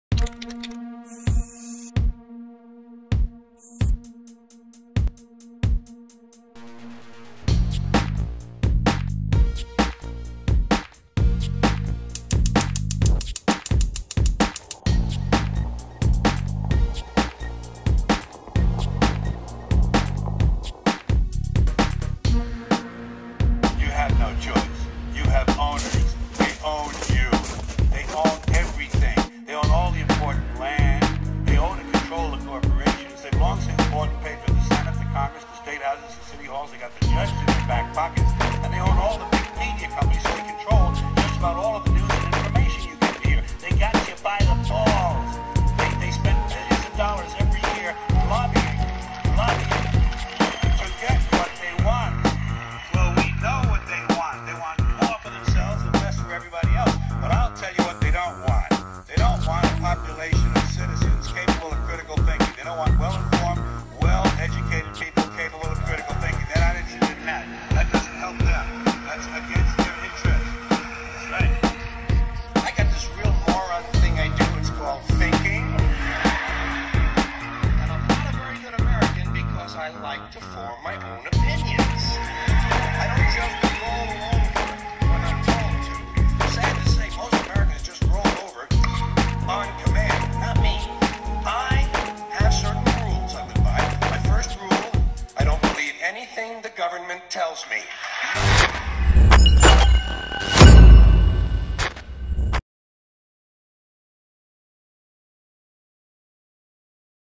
Painting Graffiti on Trains with Vhue and Infamy Art
The Infamy Art team spent some time painting graffiti on trains with artist Vhue using Loop Spray Paint. Whether your curious what a graffiti artist looks like in his natural habitat, or your looking for some insight into the process of painting trains, we are sure you'll enjoy the video.

Loop spray paint and all other art supplies used in this video are available at infamyart.com!